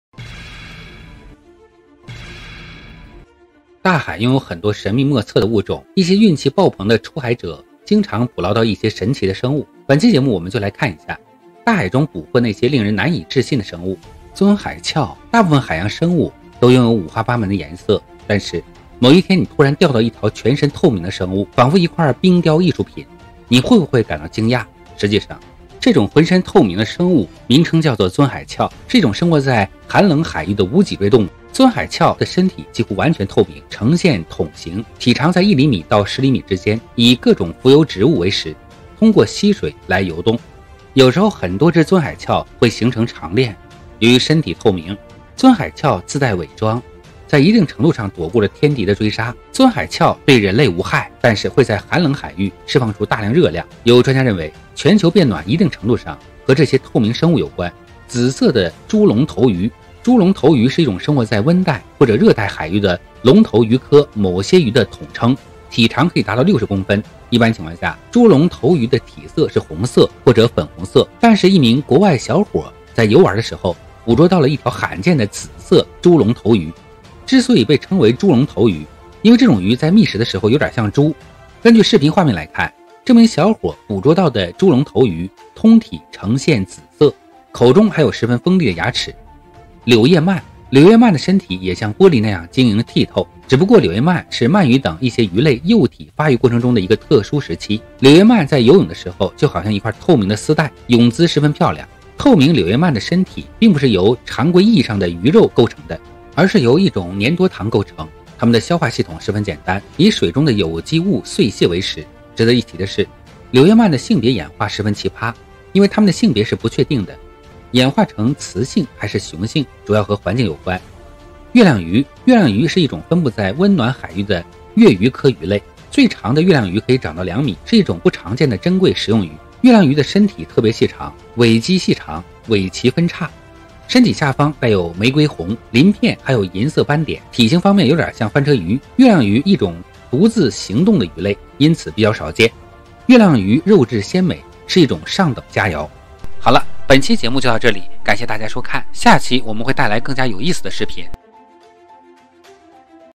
大海中捕获的4种神奇生物，海洋深处充满未知
大海拥有很多神秘莫测的物种
一些运气爆棚的出海者经常捕捞到一些神奇的生物
本期节目我们就来看一下
大海中捕获的那些令人难以置信的生物